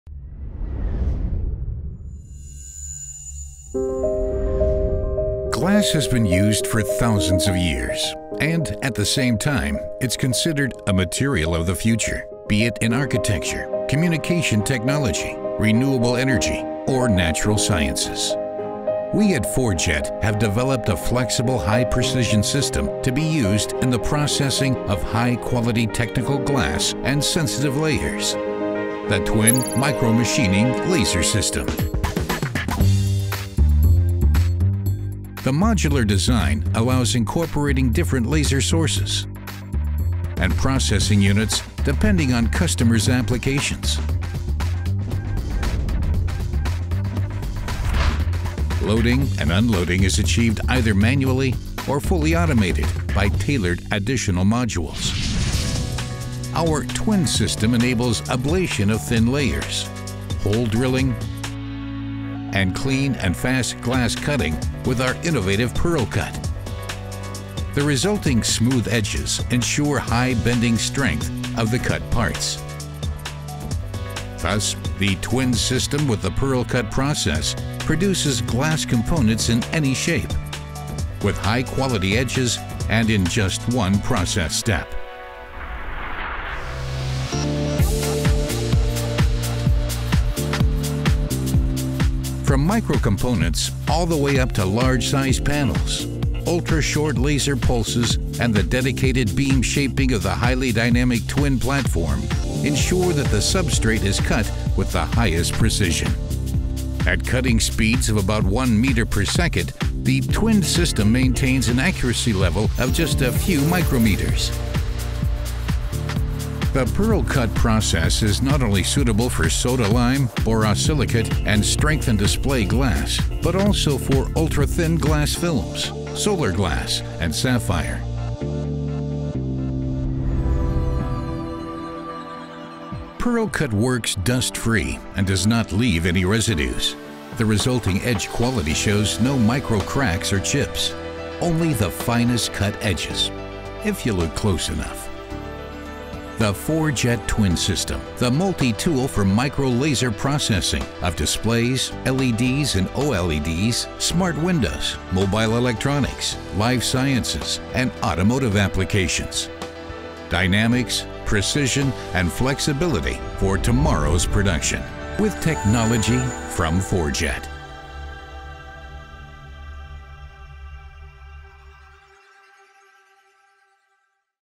2017 Laser Glass Processing by 4JET - Cutting, Drilling, Patterning with the TWIN
Laser cutting, drilling or patterning glass or films with perfect quality - fast, flexible, cost efficient and with highest precision. There are hardly enough buzzwords to introduce our new TWIN Laser Micromachining Station!  Equipped with our innovative PearlCut process the TWIN machine allows to cut glass in any shape, without debris and with "cutting edge edges". The dual head design allows to integrate additional processing steps, such as thin-film ablation of ITO or other conductive oxides, metals or semiconductors,. The super precise all granite platform allows to integrate up to 3 laser sources in one machine. CCD camera allignment for fiducials, automated calibration and a powerful and easy to use import routine for CAD files make the TWIN the versatile solution for your micrmachining needs.